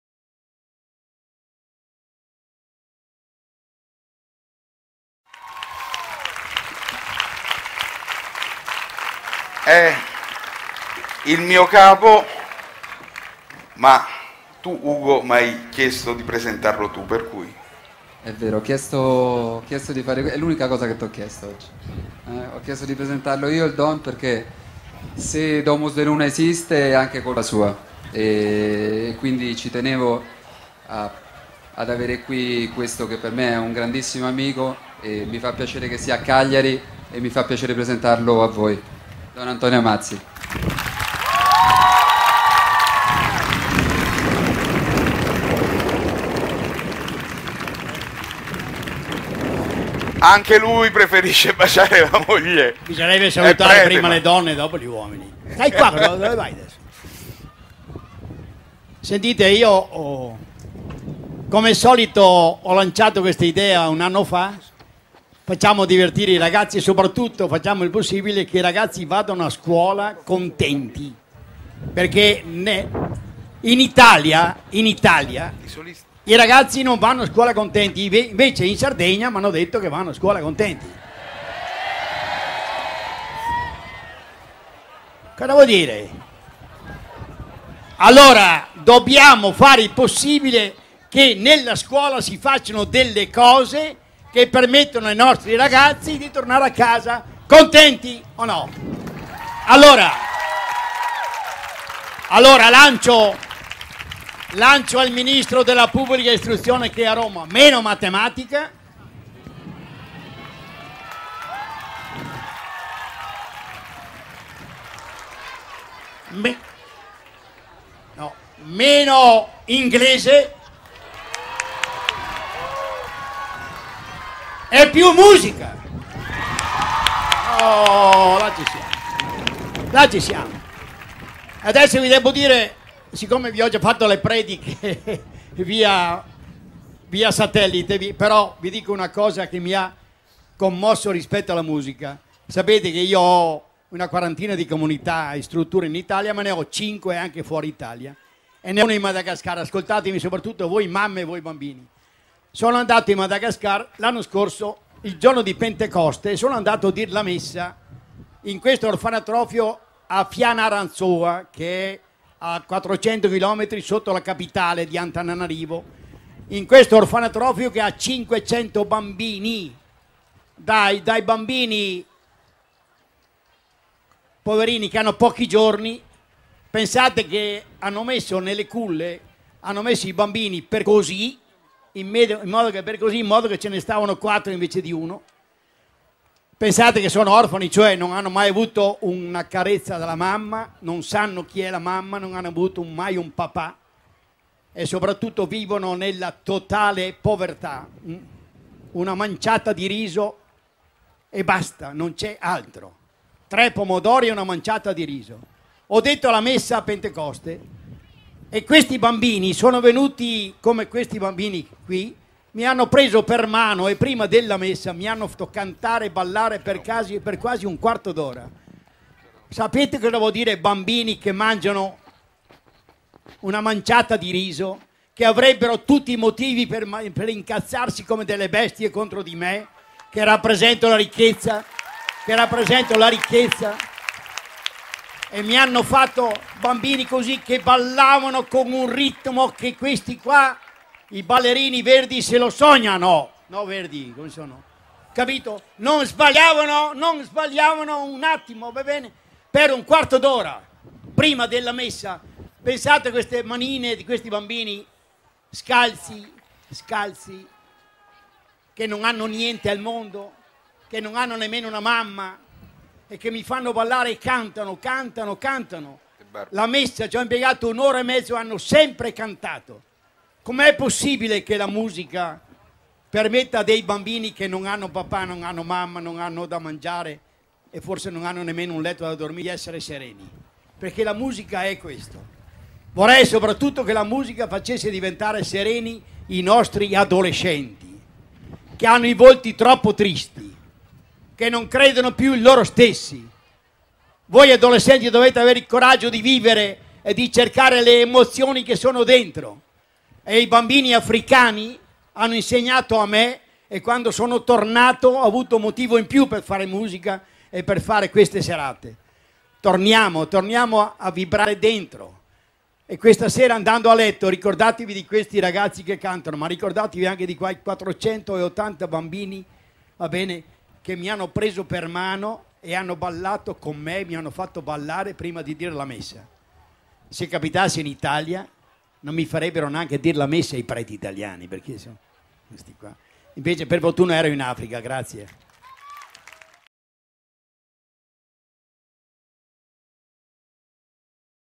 GRAFFITI MUSIC - L'intervento di Don Antonio Mazzi
In questa intervista Don Antonio Mazzi parla di come vorrebbe la scuola oggi